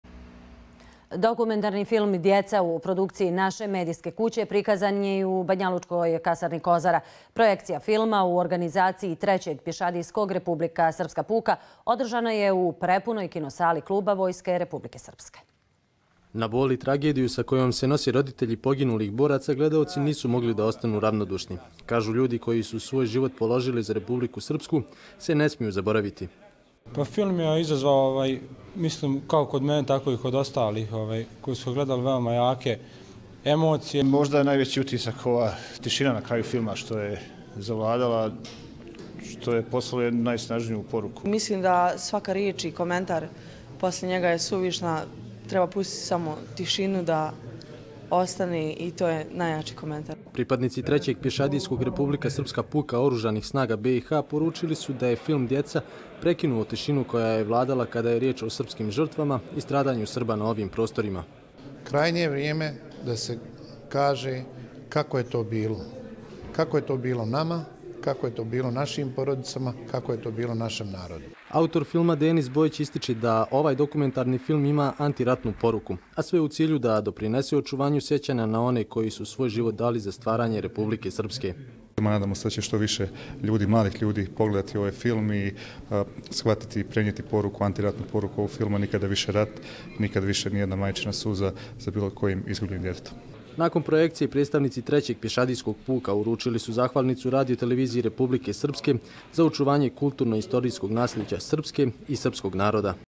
Dokumentarni film "Djeca" prikazan u Kasarni Kozara u Banjaluci
Dokumentarni film "Djeca" autora Denisa Bojića prikazan je danas u Kasarni Kozara, u Banjaluci. Projekcija filma, u organizaciji Trećeg pješadiskog Republika Srpska puka, održana je u prepunoj kino - sali Kluba Vojske Republike Srpske.